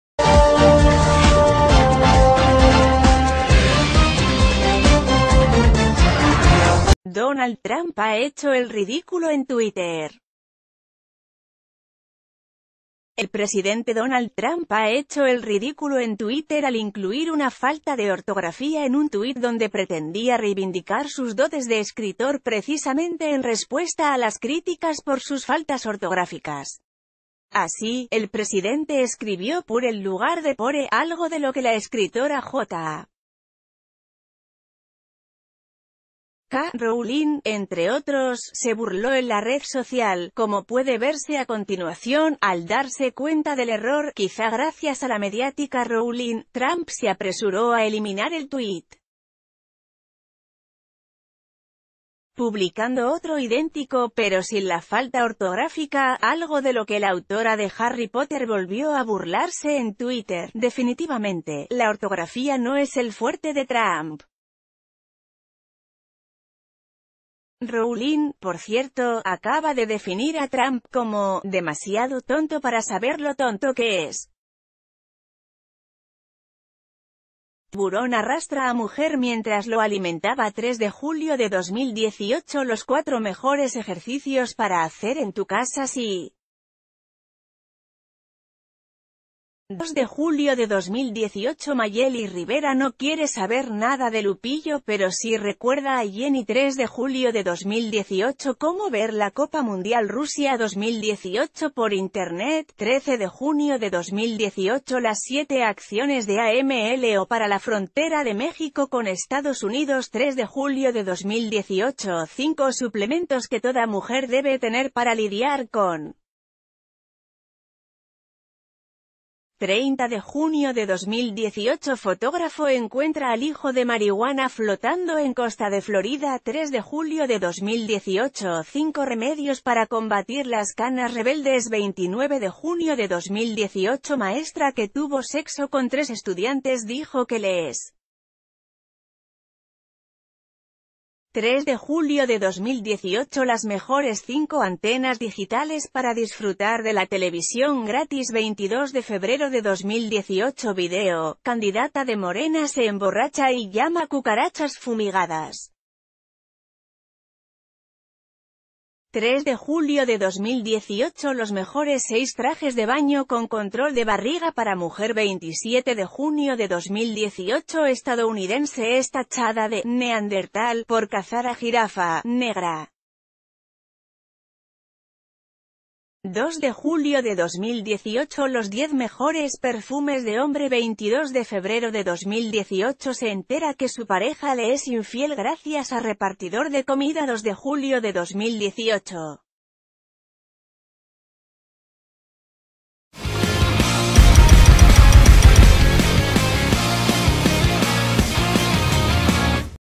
Trump presume de buena ortografía… ¡En un tuit con faltas!
Donald Trump ha hecho el ridículo en Twitter. El presidente Donald Trump ha hecho el ridículo en Twitter al incluir una falta de ortografía en un tuit donde pretendía reivindicar sus dotes de escritor precisamente en respuesta a las críticas por sus faltas ortográficas. Así, el presidente escribió p...